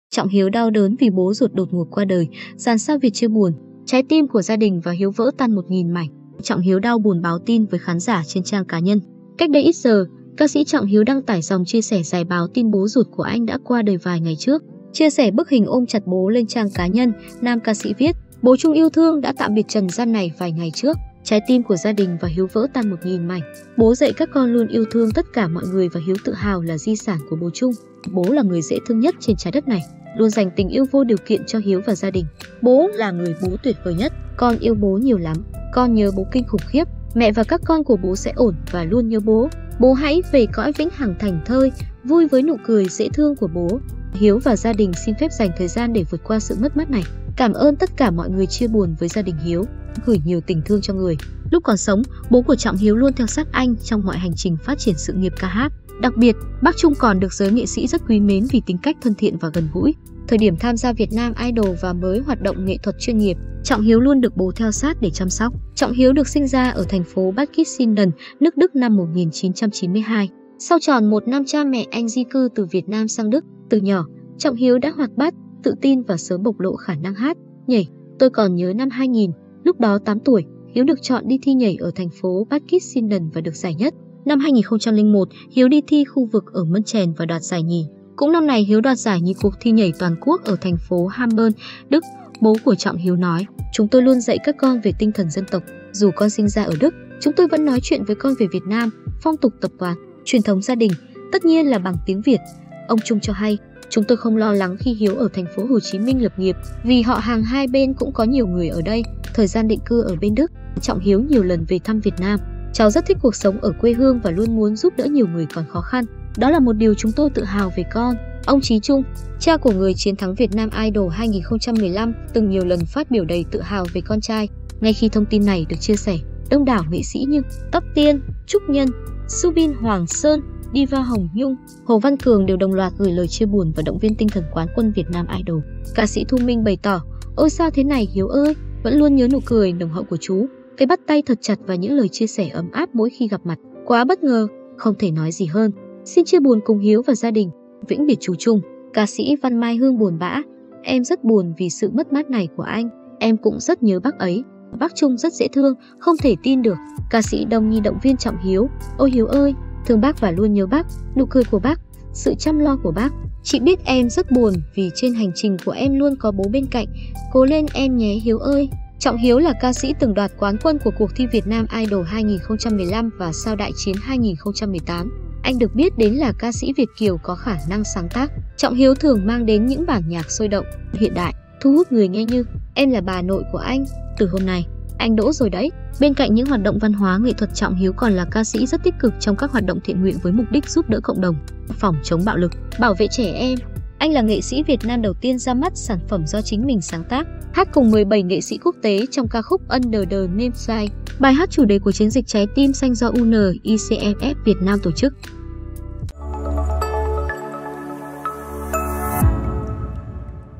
Trọng Hiếu đau đớn vì bố ruột đột ngột qua đời, dàn sao Việt chia buồn
Cách đây ít giờ, ca sĩ Trọng Hiếu đăng tải dòng chia sẻ dài báo tin bố ruột của anh đã qua đời vài ngày trước. Chia sẻ bức hình ôm chặt bố lên trang cá nhân, nam ca sĩ viết: "Bố Trung yêu thương đã tạm biệt trần gian này vài ngày trước. Trái tim của gia đình và Hiếu vỡ tan 1000 mảnh".